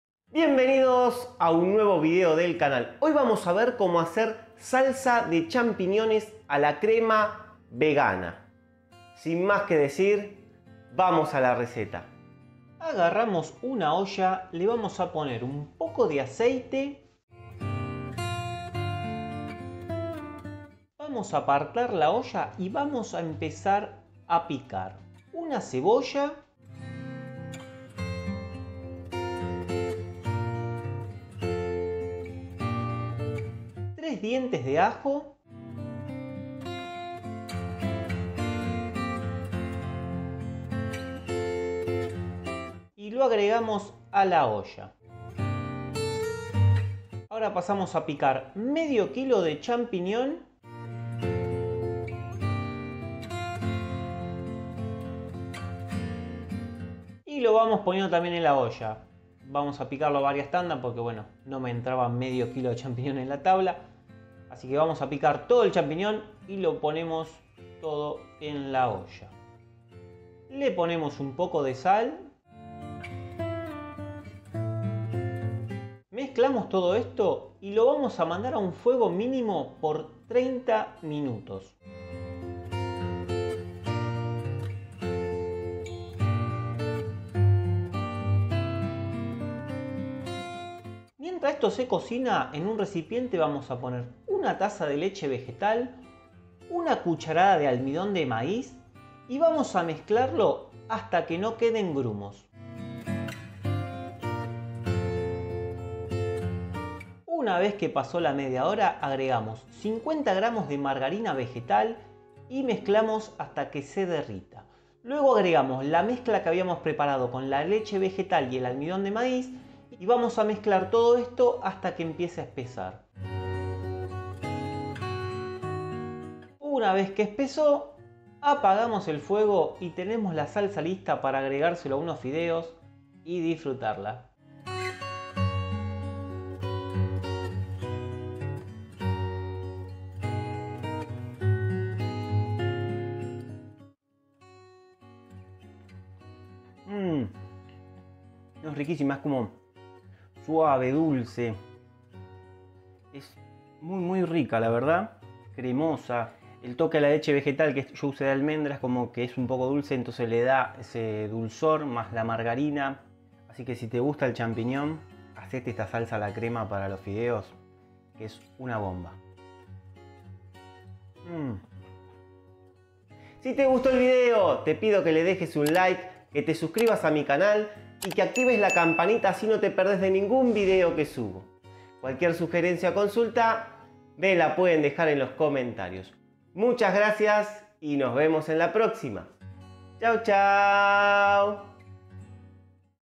SALSA DE CHAMPIGNON A LA CREMA VEGANA
Ingredientes:

- 1 Cebolla.
- 3 Dientes de ajo.
- 1/2 Kg de champignon.
- Sal.
- 1 Taza de leche vegetal.
- 1 Cucharada de almidón de maíz.
- 50 Gr de margarina vegetal.